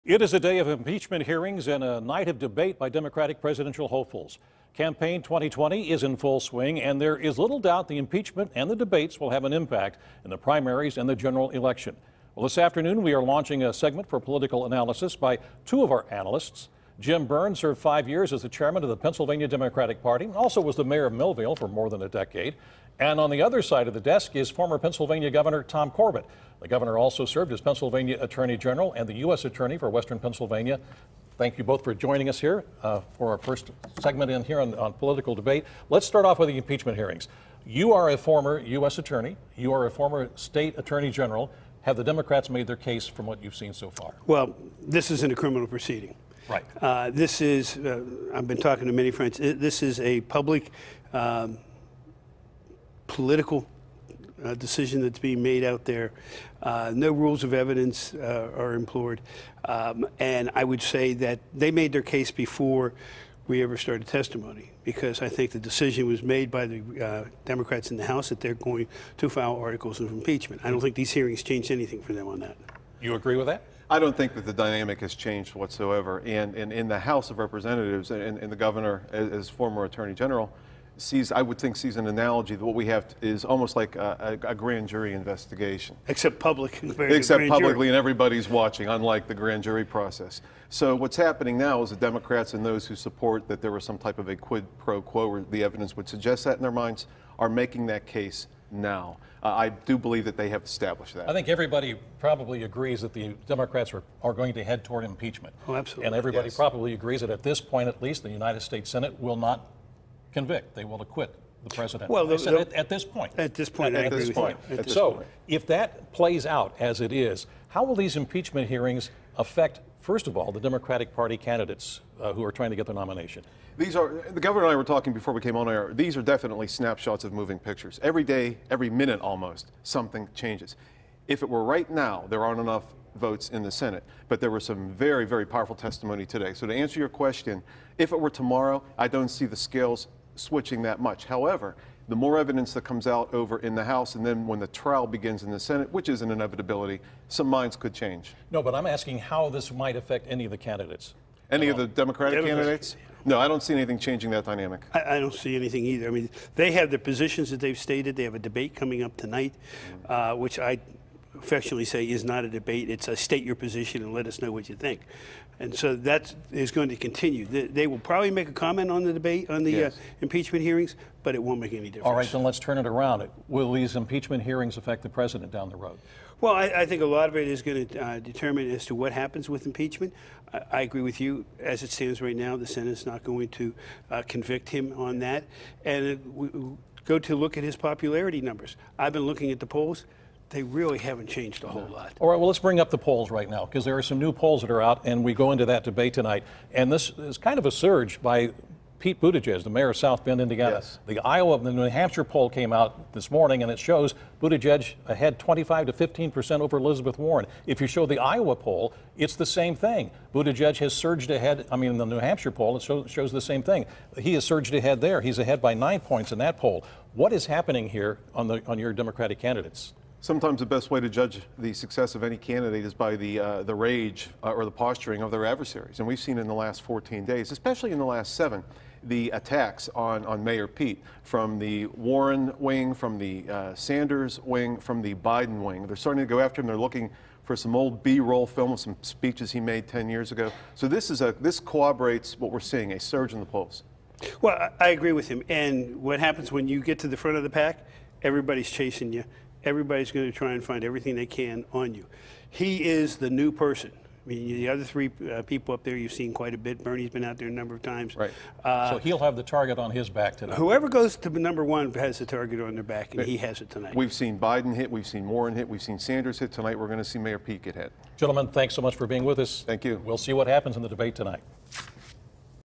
Political Interview: Impeachment And 2020 Election
Former PA Democratic Party Chairman Jim Burn and former PA Governor Tom Corbett join KDKA's Stacy Smith to talk politics.